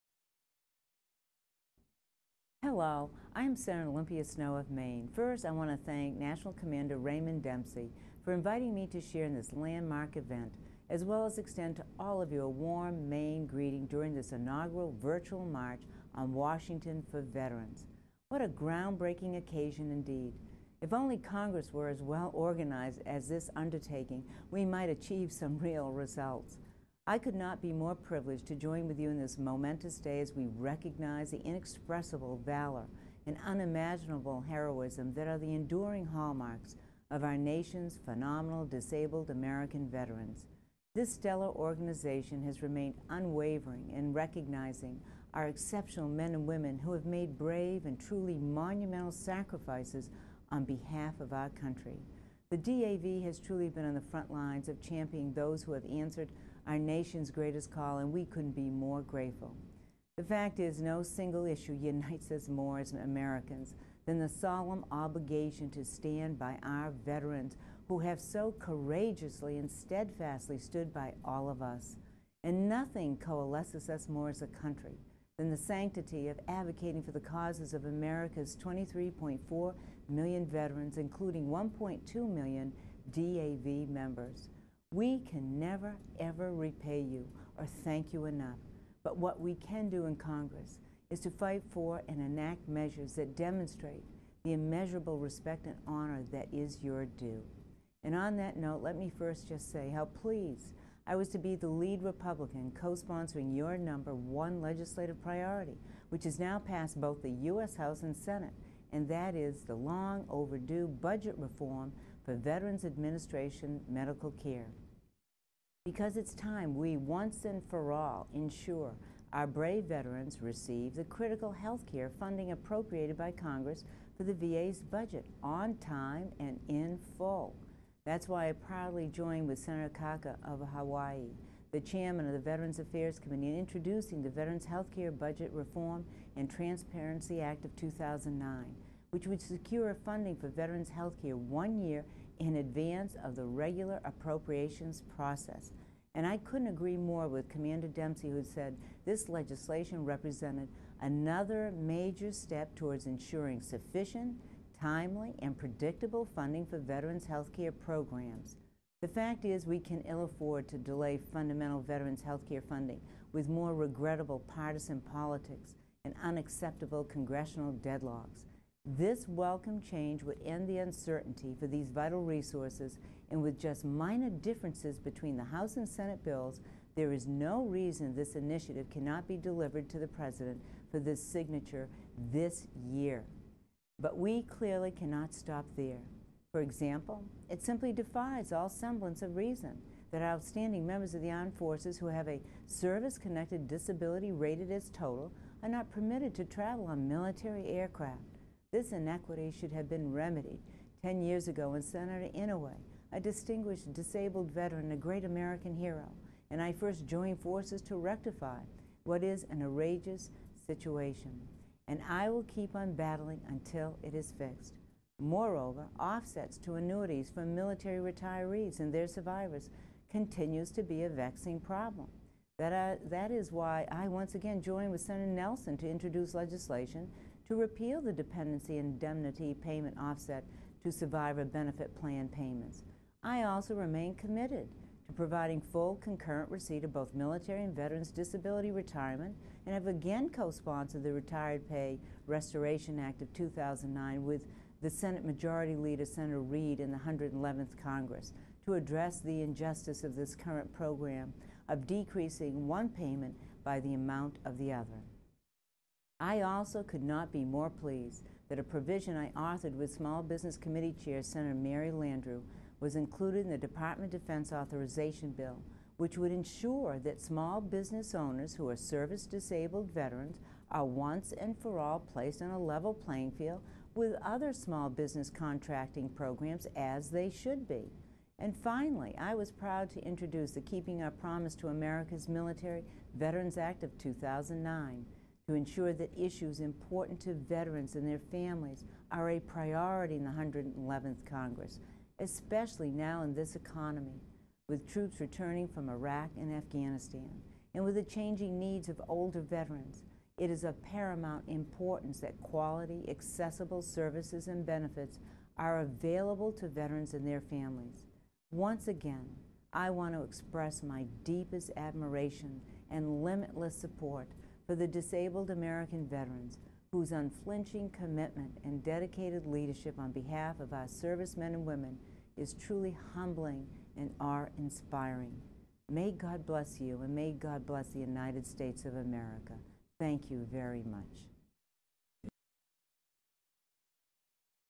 Sen Olympia Snowe Virtually Marches for Veterans
United States Senator Olympia J. Snowe (R) for Maine.